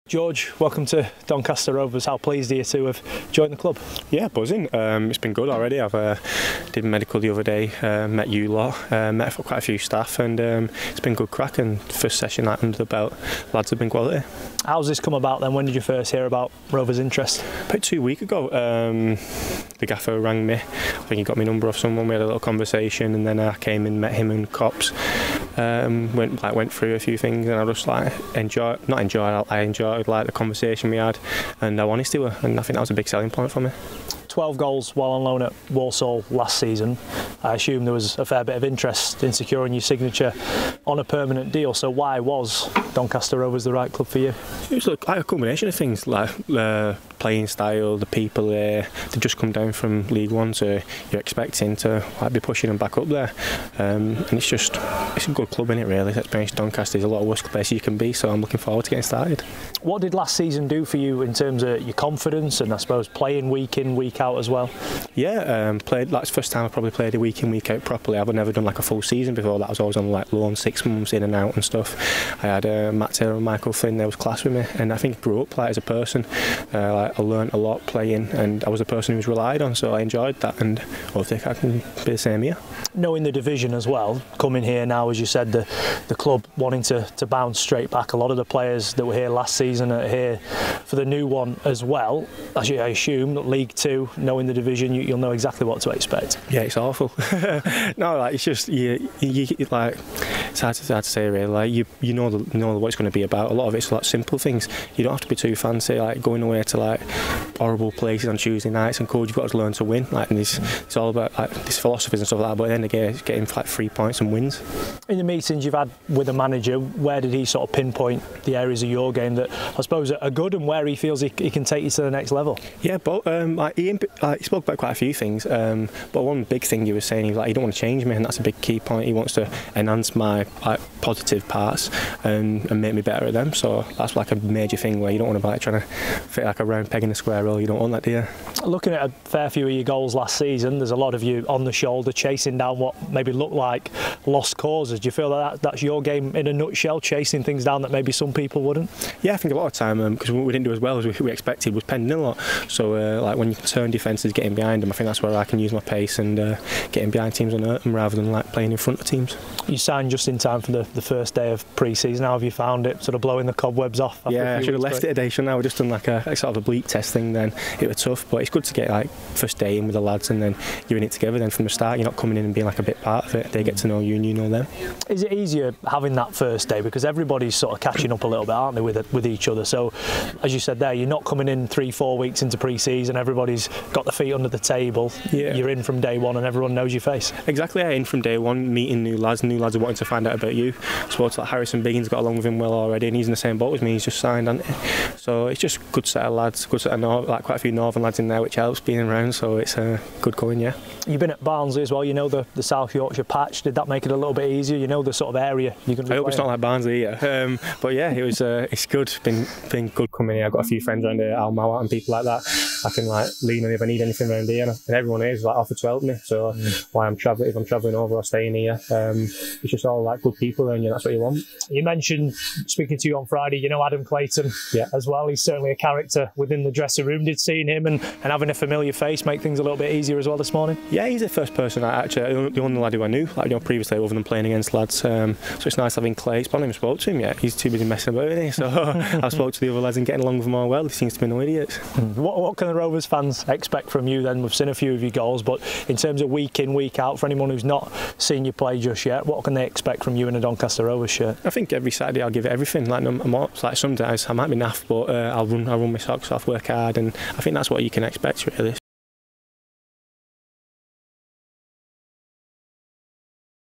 Rovers' new number nine...George Miller!
Rovers are delighted to announce the signing of striker George Miller on a three-year deal. Here, he reflects on his arrival at the club and gives an insight into what he will bring to the squad.